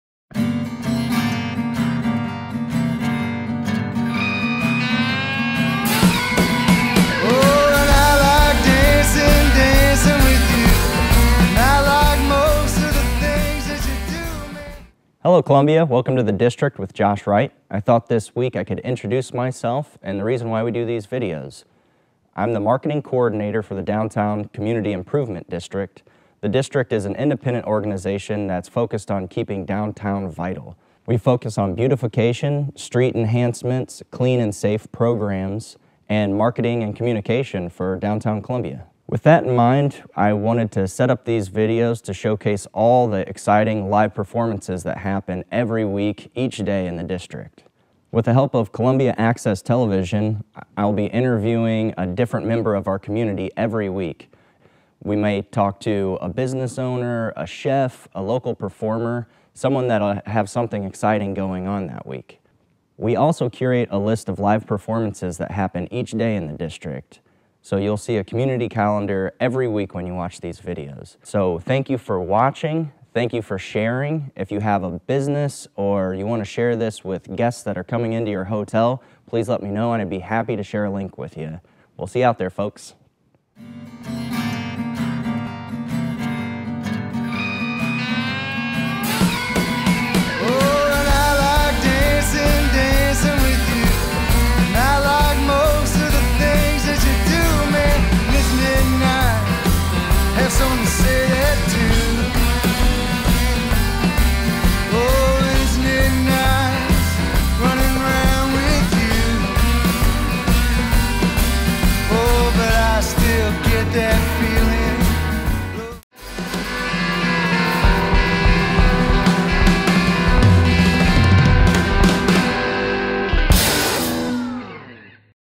The District with Josh Wright Ep7
Josh explains the Downtown Community Improvement District and what this video series is all about. Plenty of events activities this week in Columbia, Missouri.